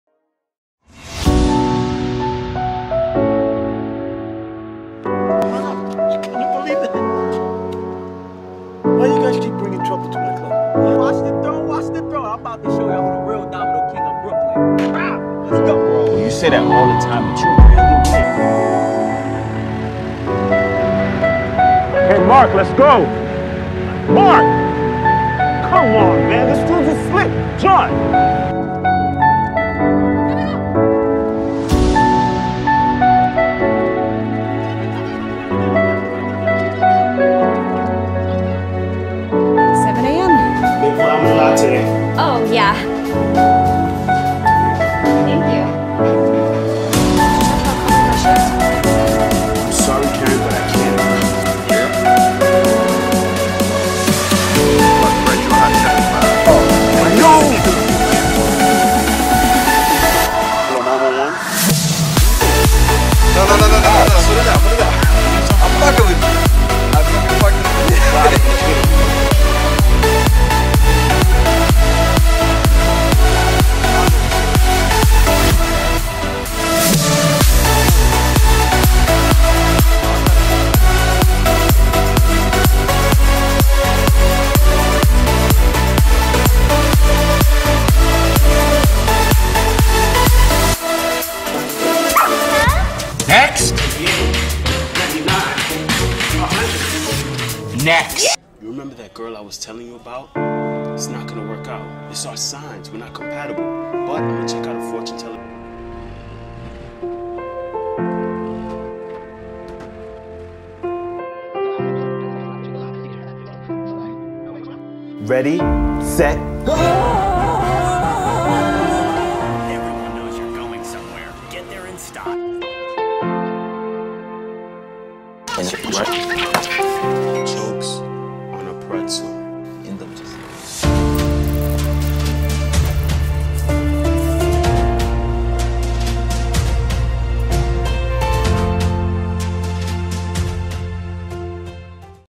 Eric Arriola - An Actor's Life
Just a quick snapshot of my acting career. Truly grateful for every moment and every opportunity and the thing is I am just getting started. Have you ever acted in anything?